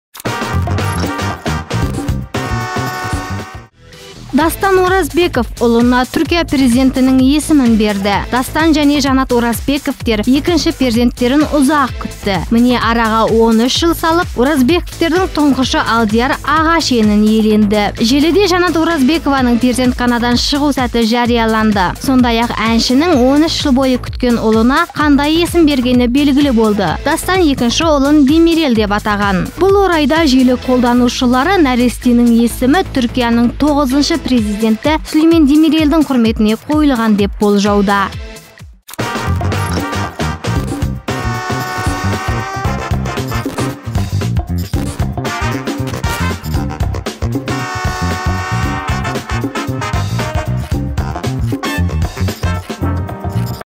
Дастан Оразбеков екінші ұлына Түркия президентінің атын берді
Дастан Оразбеков ұлына Түркия президентінің есімін берді. Дастан және Жанат Оразбековтер екінші перзенттерін ұзақ күтті. Міне, араға 13 жыл салып, Оразбековтердің тұңғышы Алдияр “аға” шенін иеленді.